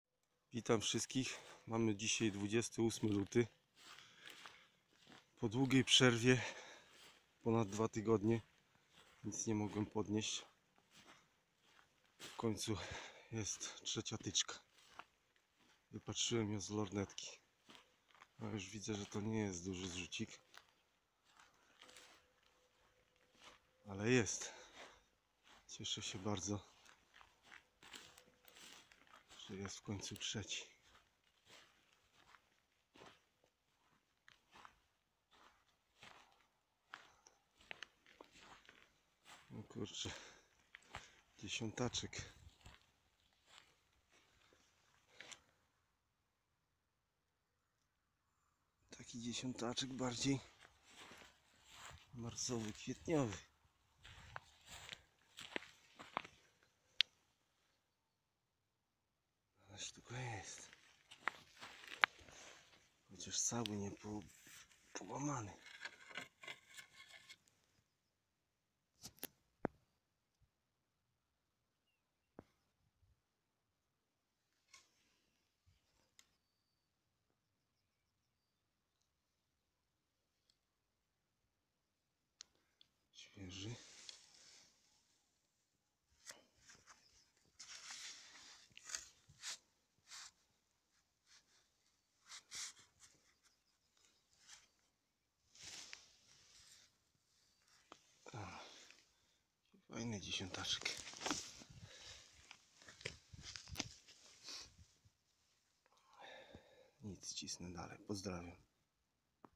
Zrzuty 2020. 12-Stak #9
Przeprasza z góry za 10-taka bo to jest 12-stak,ale z emocji zile obliczyłem:) Trzecia tyczka w tym sezonie się trafiła po 2 tygodniach pustki.. Mały 12-tak ale cieszy:)